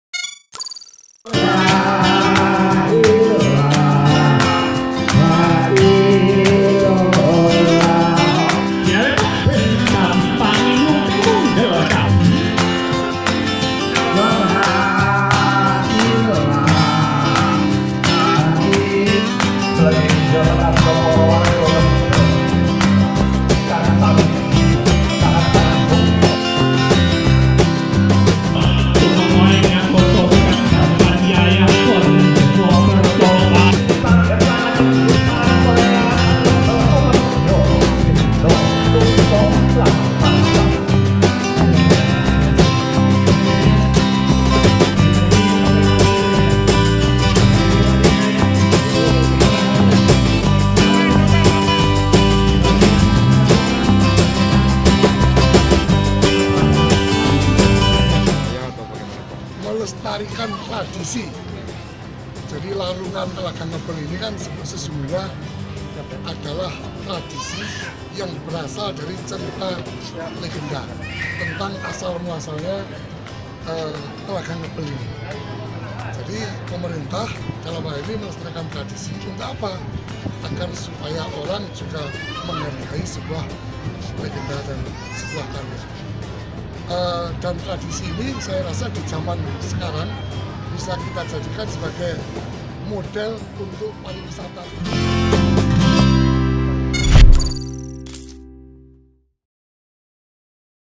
Meriahnya Tradisi Larung Sesaji di Ponorogo
Warga Ponorogo menggelar tradisi larung sesaji di Telaga Ngebel Ponorogo, Jawa Timur. Tradisi ini merupakan tradisi tahunan yang dilakukan secara turun temurun.